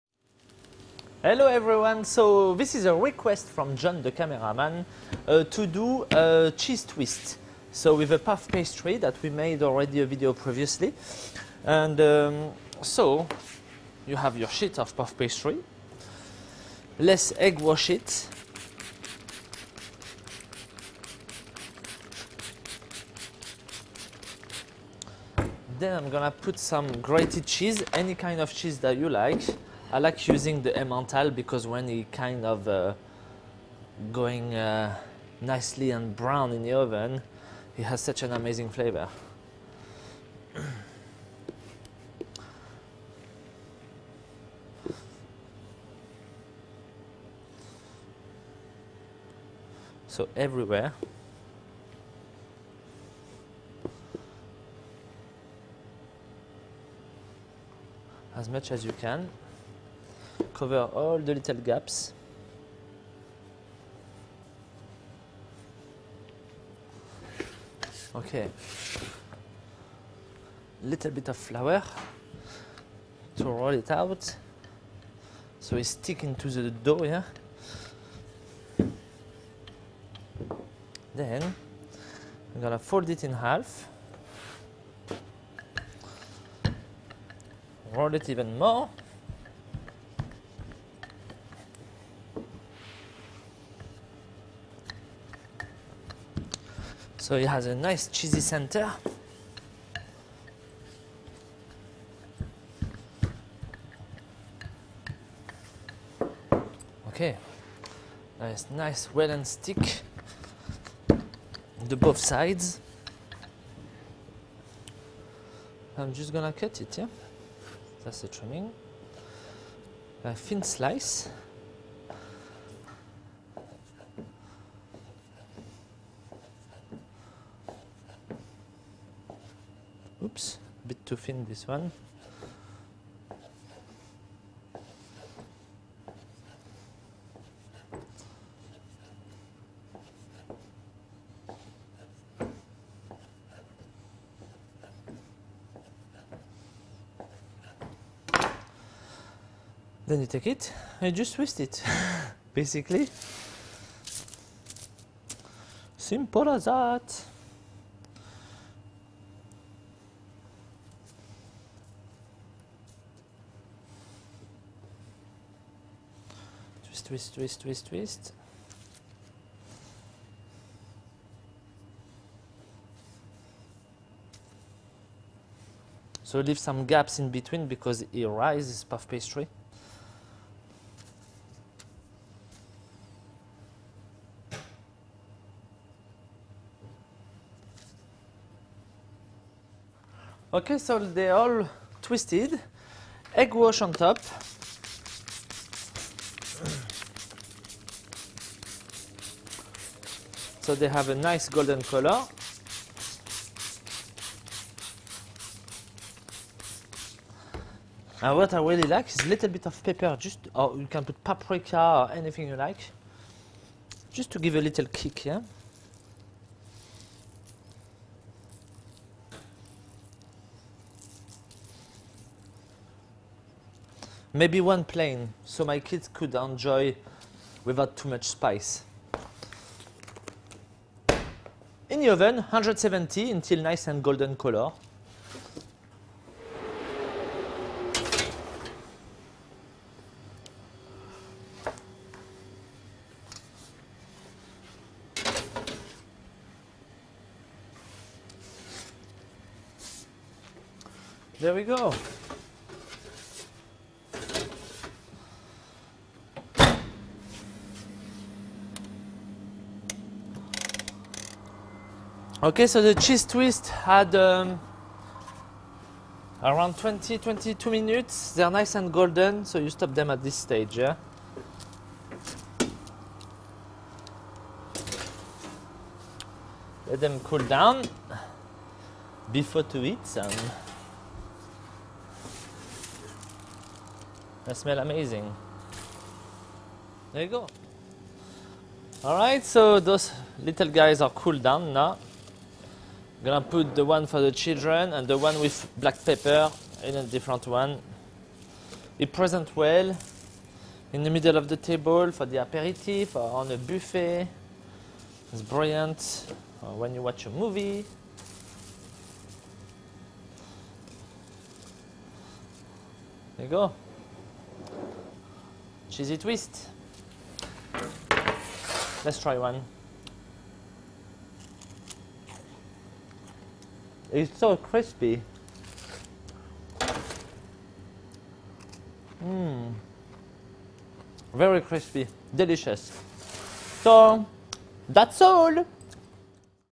How to make Cheese Twists with award winning baker in the South West French T.V. Chef Julien
Cheese Twists with a puff pastry with French Chef Julien Picamil from Saveurs Dartmouth U.K.
Ingredients / Puff Pastry and Cheese of your choice,